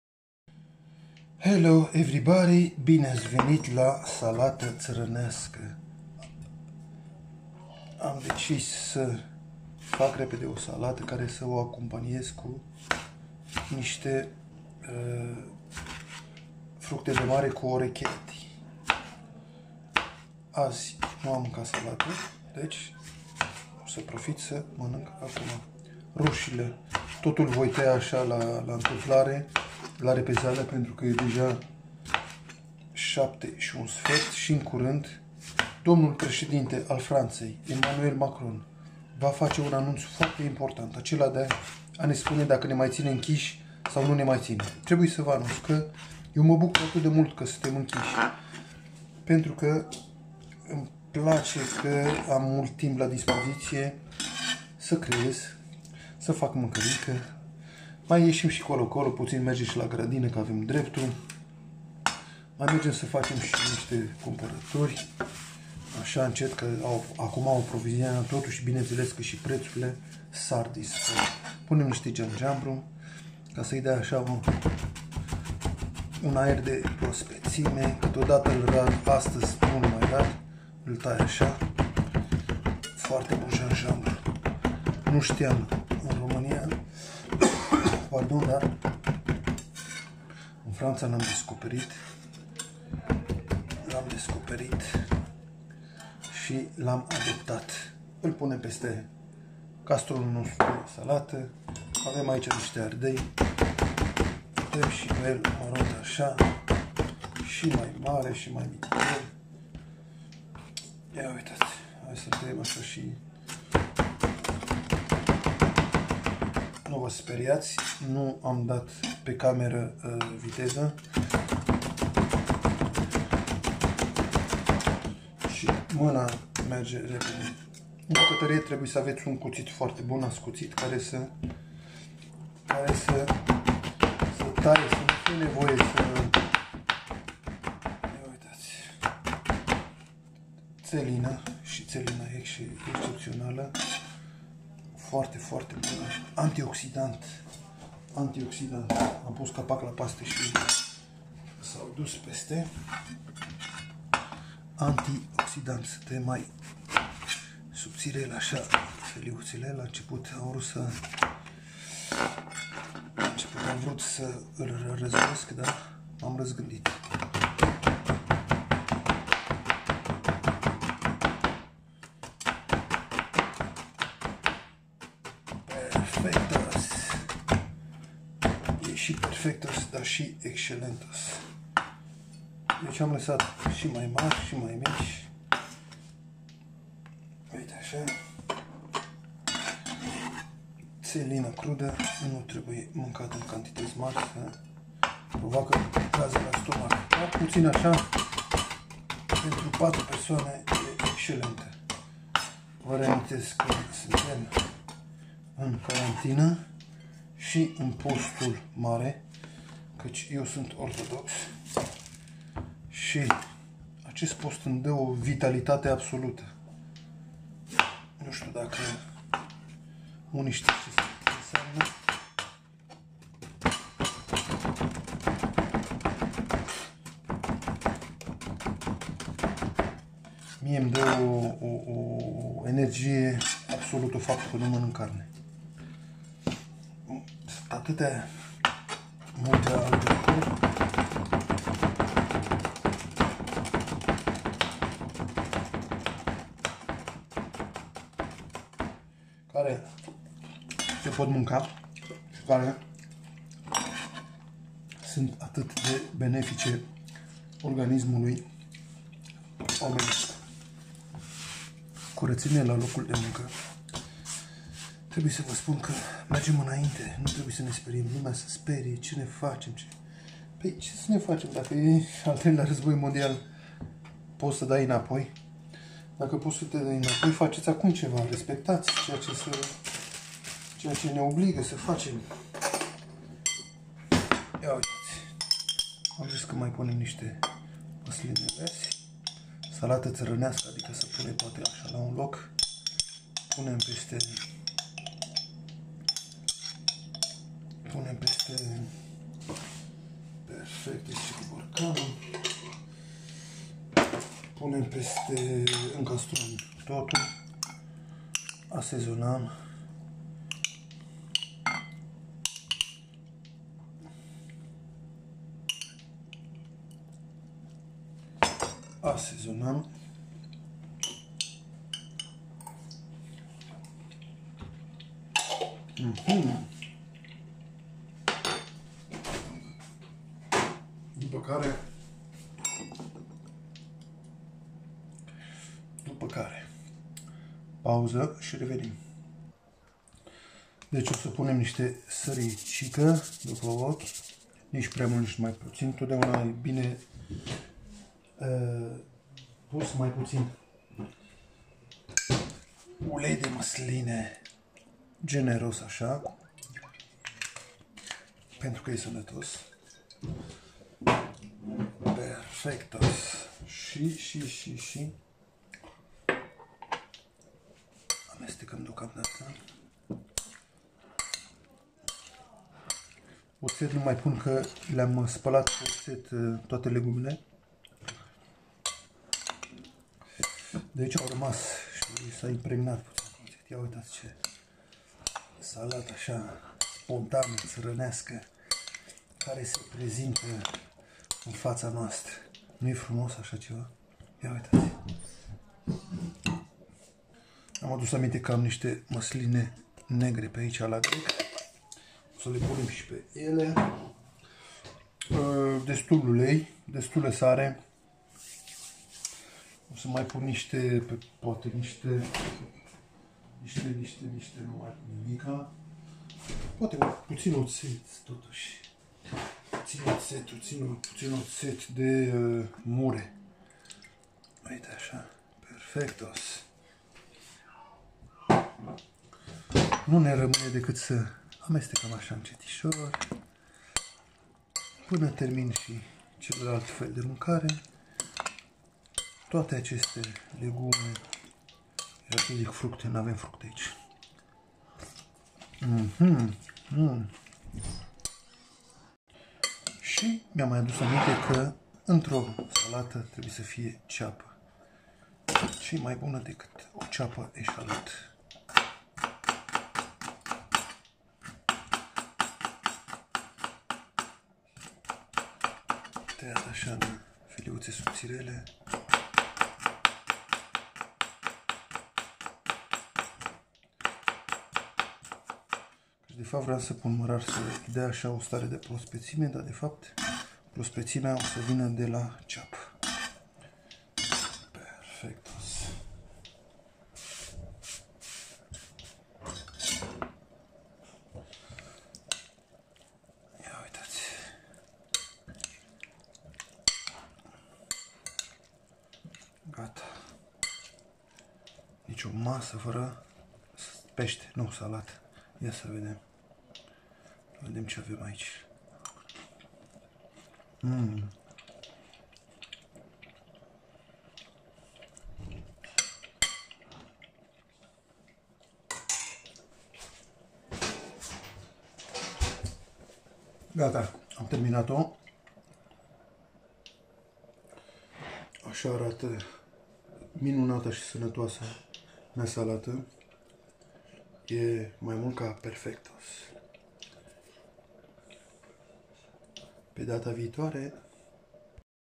SALATĂ DE VARĂ.
Sursă de vitamine, salată țărănească de crudități este o sursă de vitalitate și sănătate. Consumată fără reținere nu poate decât să ne aducă decât lucruri bune. Eu o numesc asa caci toate legumele sunt puse la un loc. In schimb a iesit Excelentos.